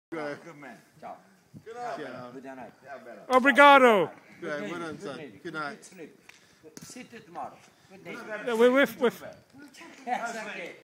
Graham horse
Graham after horse in Portugal October 09